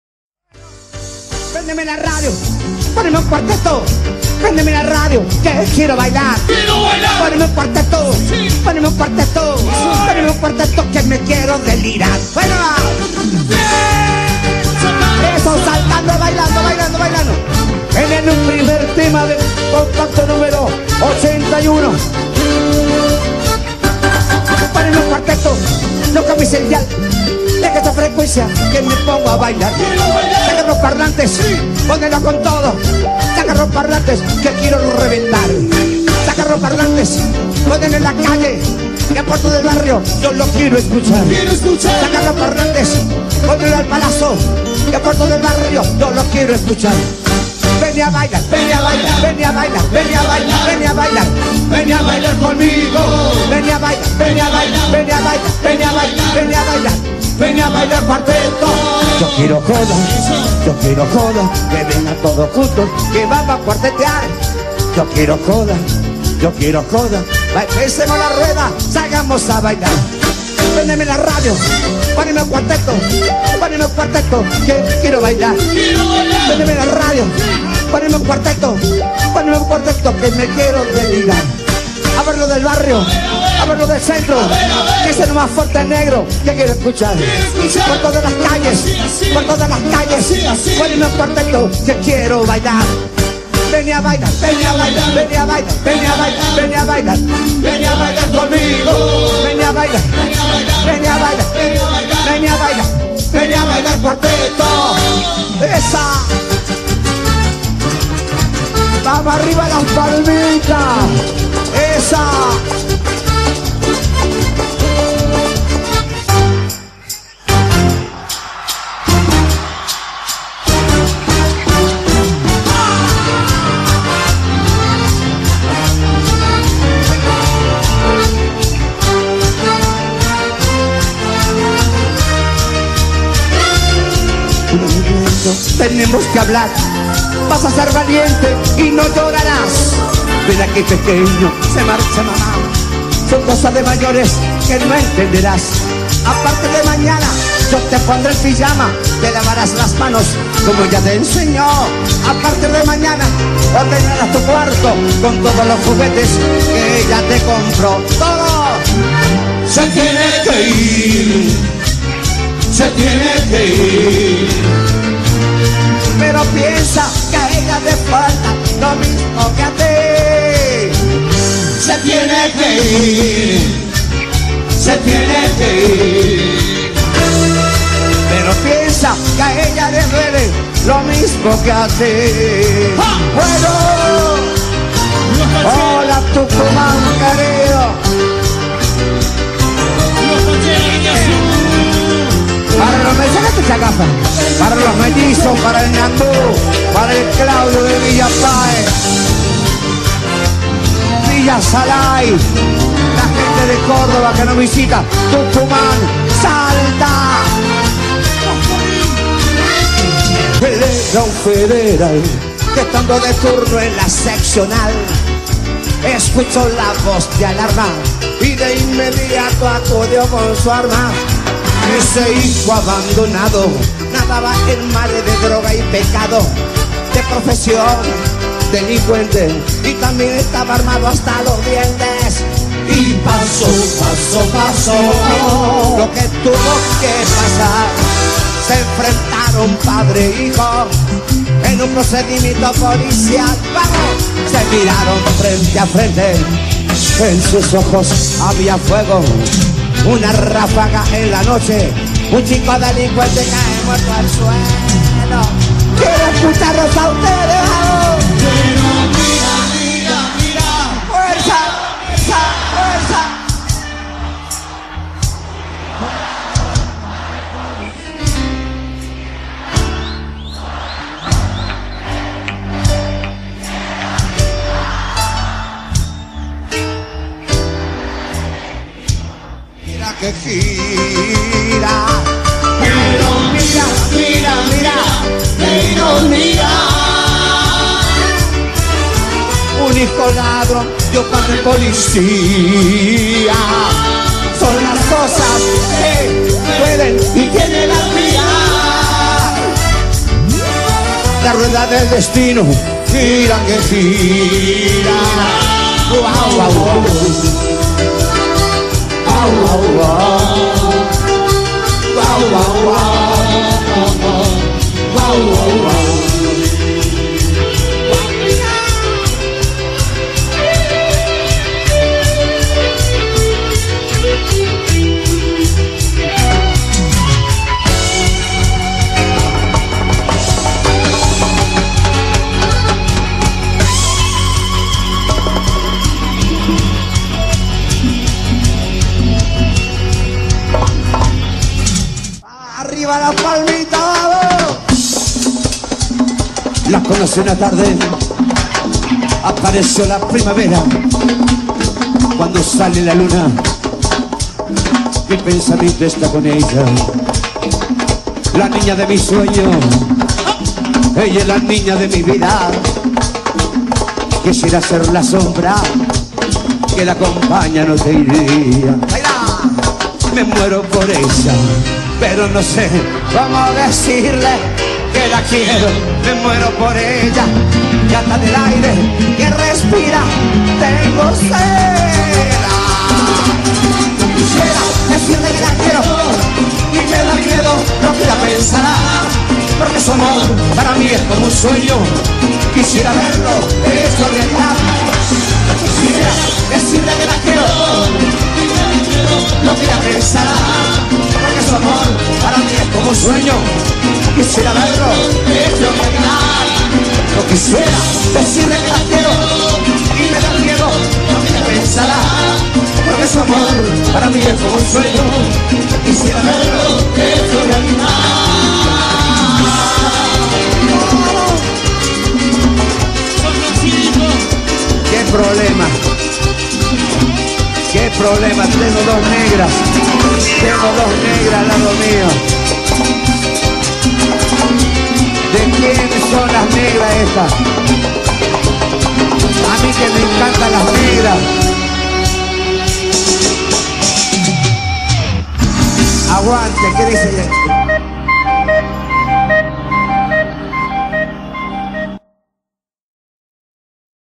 LA MONA EN VIVO RANCHILLOS 2011 (JoseeLopez)
PARA TODOS LOS CUARTETEROS, ESTOS ENGANCHADOS DEL MAS GRANDE EN VIVO EN RANCHILLOS - TUCUMAN 13 DE MARZO 2011